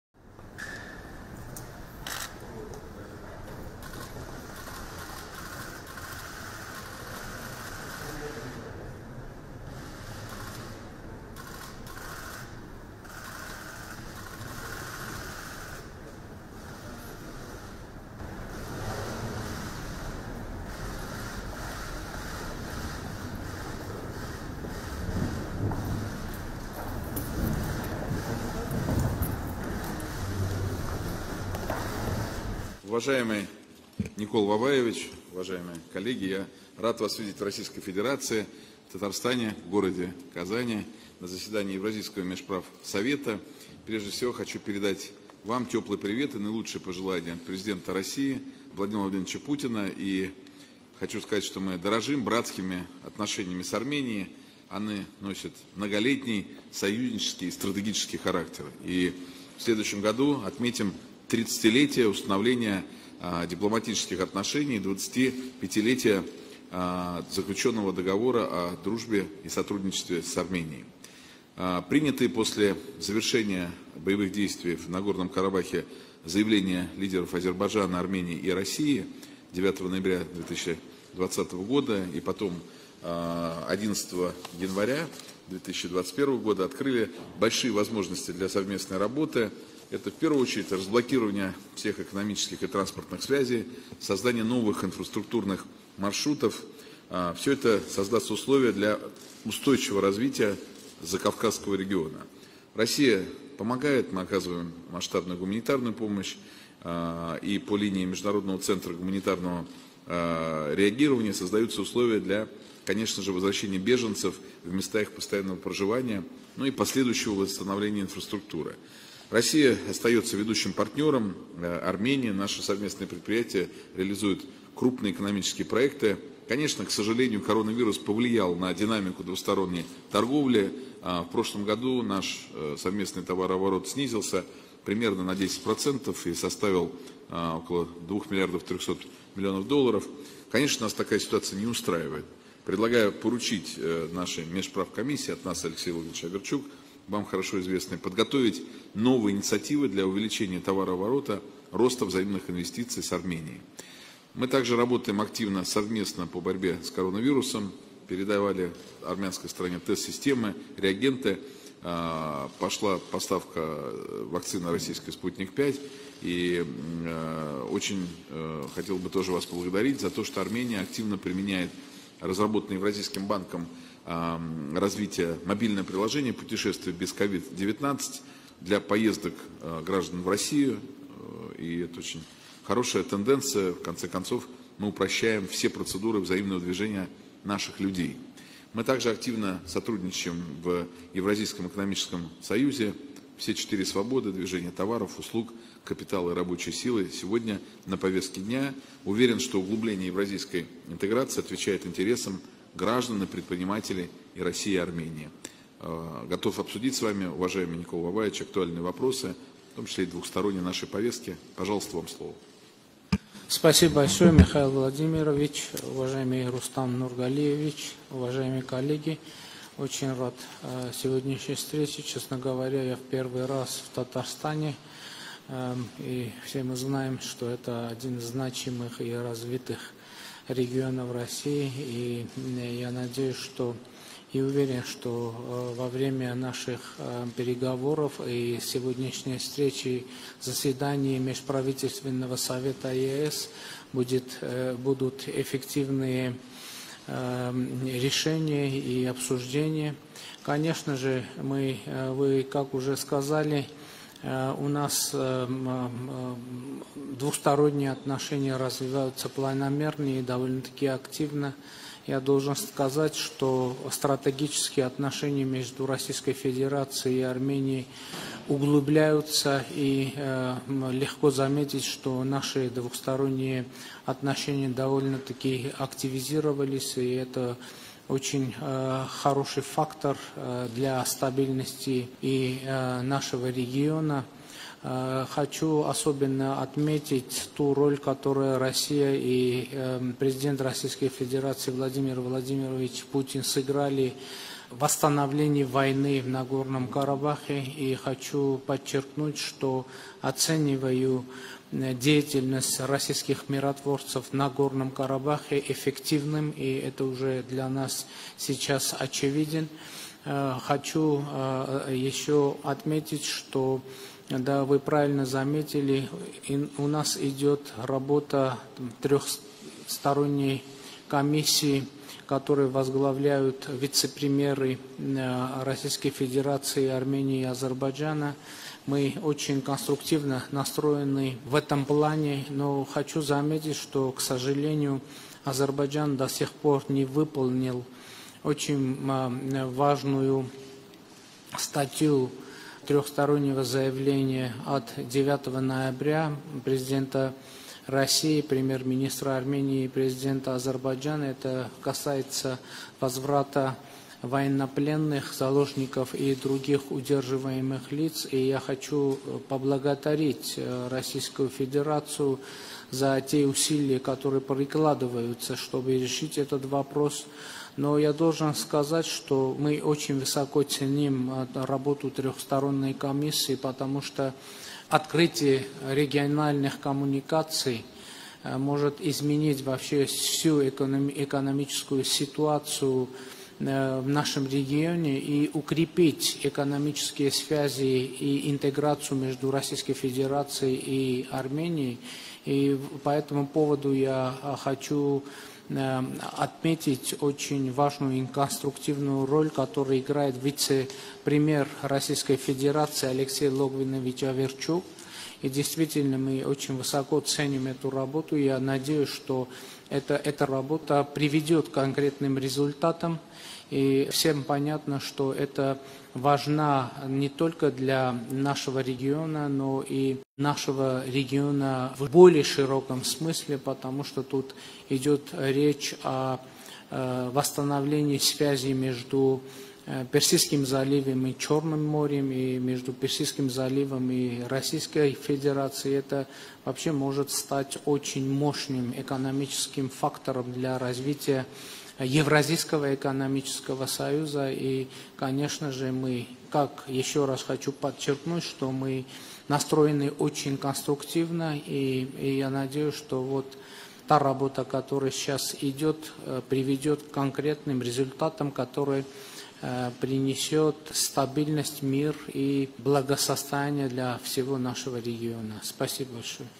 Նիկոլ Փաշինյան - Հանդիպում ՌԴ վարչապետ Միխայիլ Միշուստինի հետ։
Վարչապետս գիշեր ցերեկ աշխատում է՝ առավոտյան կառավարության նիստը վարեց հիմա էլ արդեն Կազանում է։ Հաջողությններ եմ ցանկանում